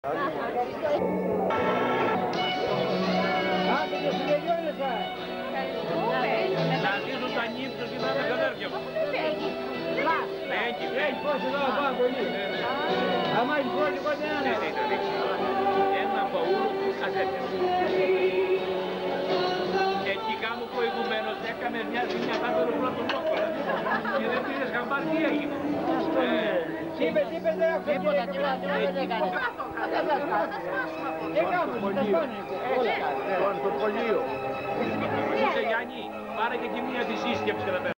17.12.1995 Γαμήλιο δείπνο του Αλέξη Μακρή και της Άννας
Διακρίνονται Μακρής Γεώργιος, η σύζυγος του Κατερίνα, ο υιός Κυριάκος video Καλύβας Ιωάννης